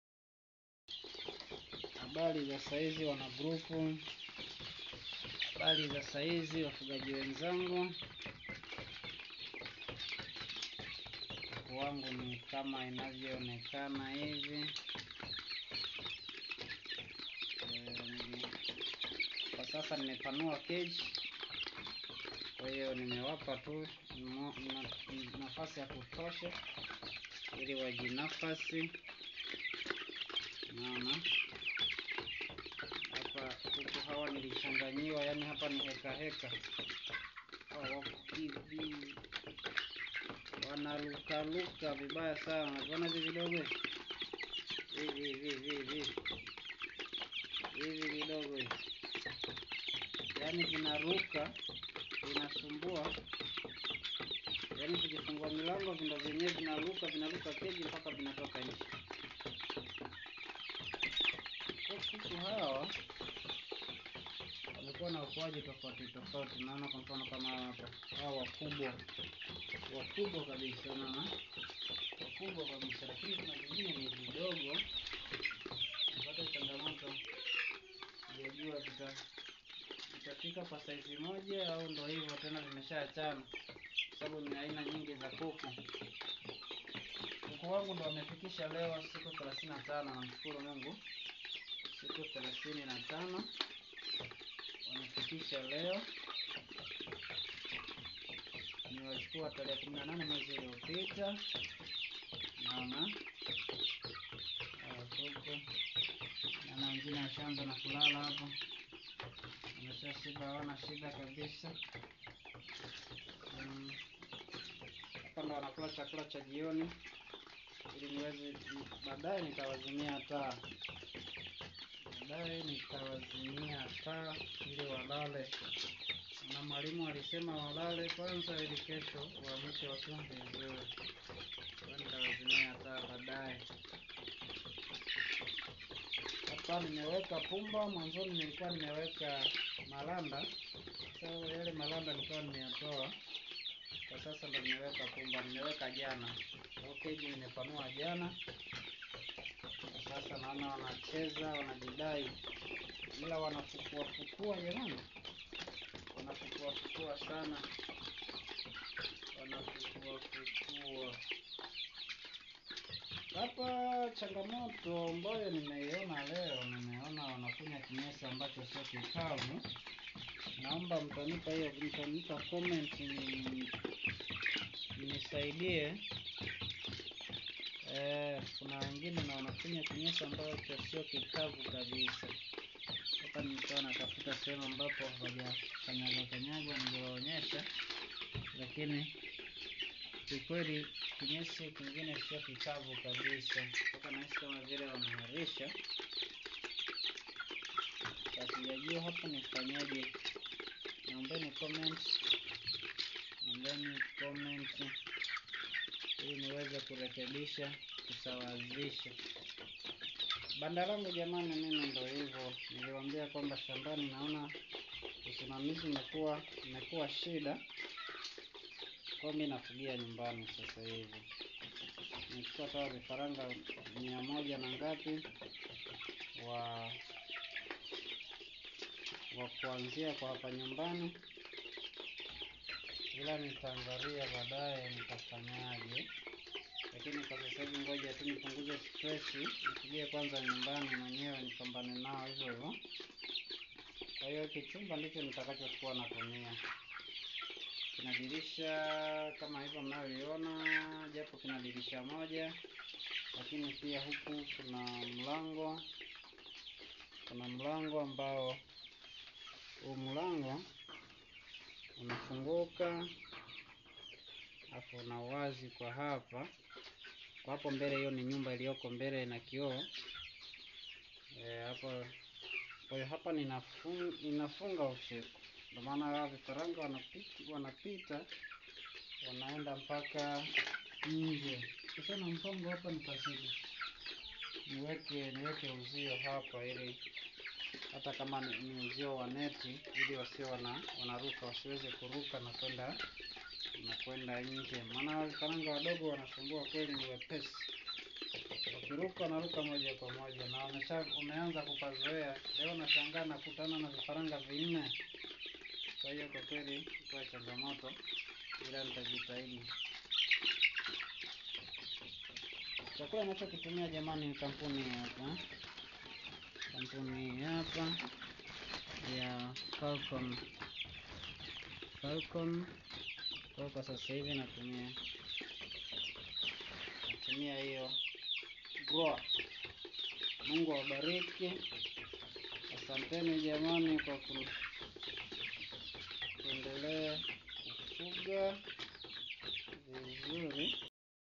KROILER KUTOKA KWA GROUP WANAFUNZI WANAJITUMA WANANITIA MOYO💪💪💪💕💕💕
Uwasilishaji guys anafuga Kroiler kwa hiyo hata makuzi take ni taratibu,, 👆